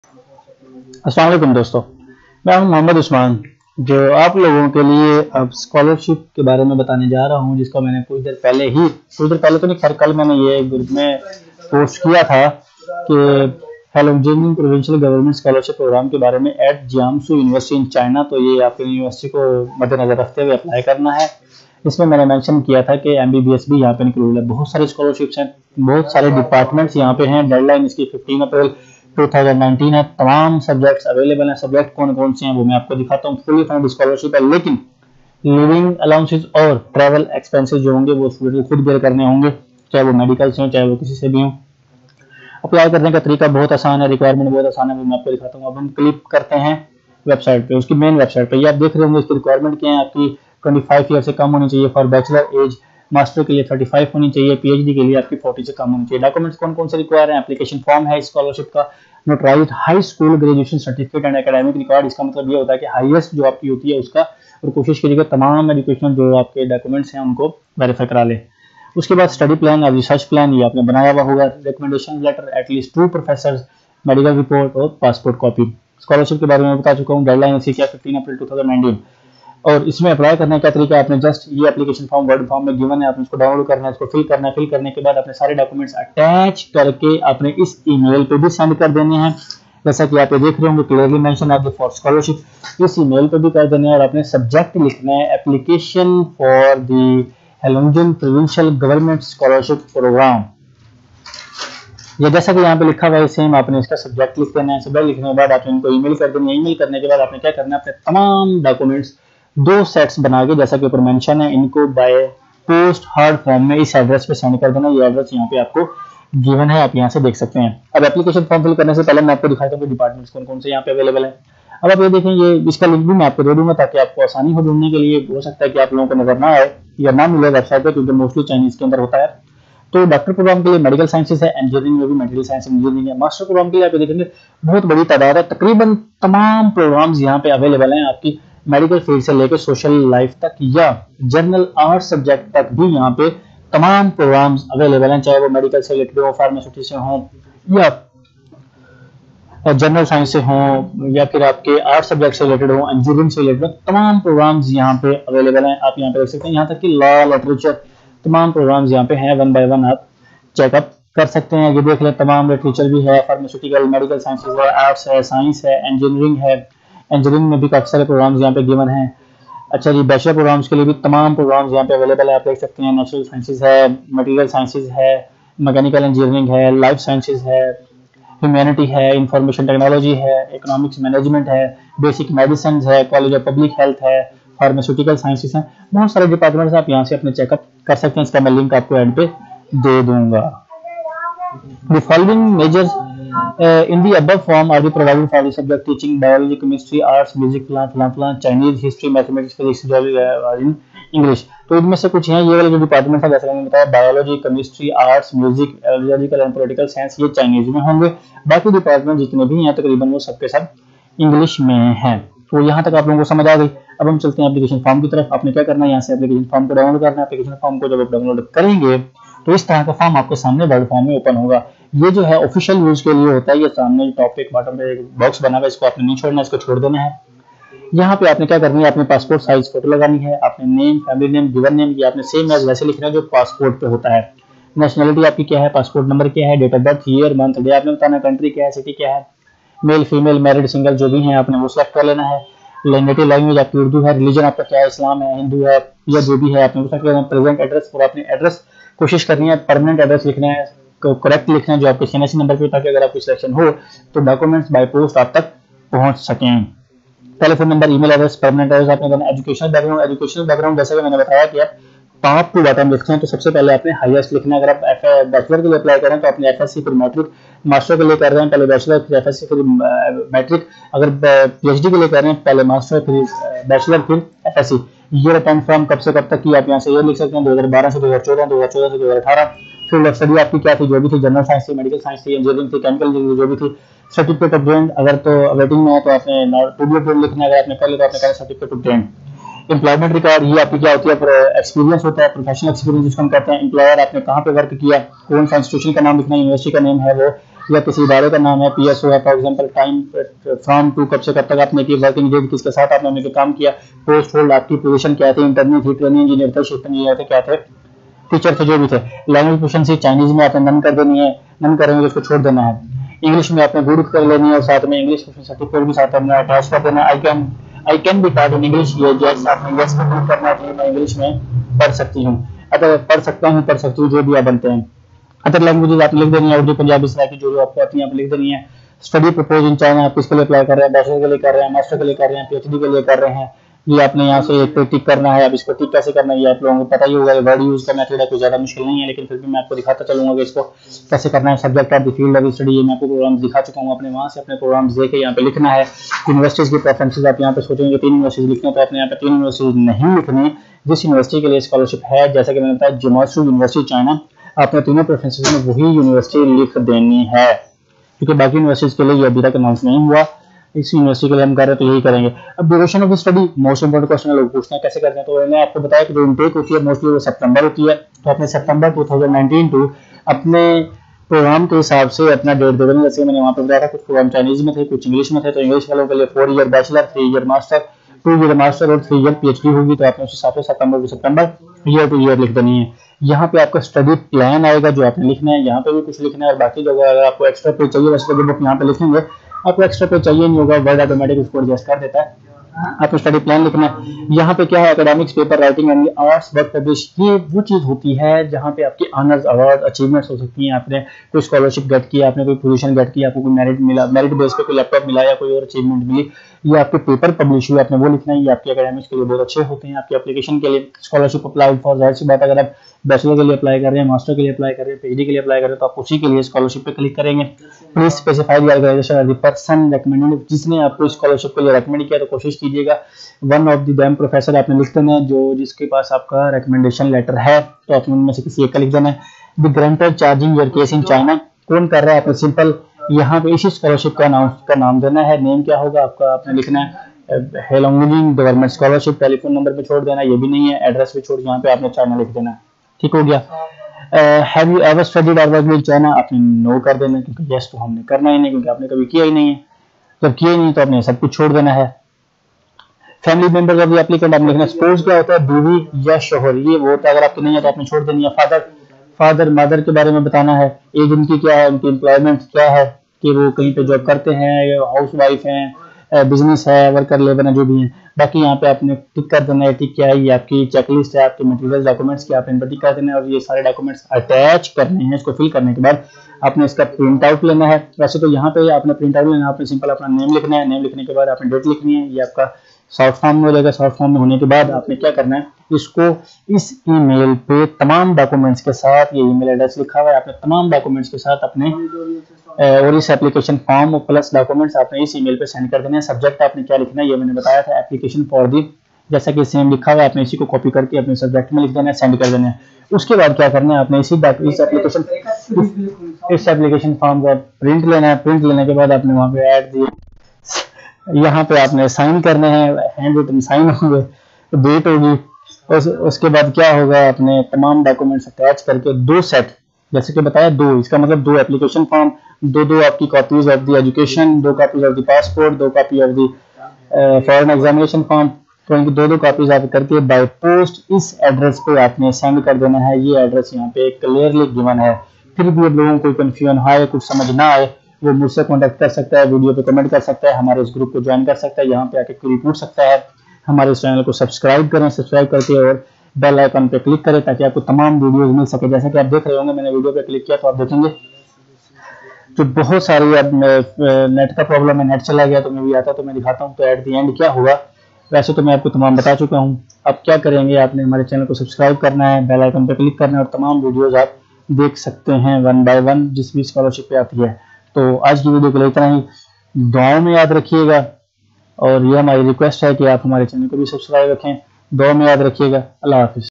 HEILONGJIANG GOVERNMENT SCHOLARSHIP 2019/2020 (Jiamusi University)
Bachelor/Master/PhD, All fields available including, Medical, MBBS, Pharmaceutical, Engineering, Life Sciences, Arts. Literature, Education, Economics, Law; Deadline: 15th April 2019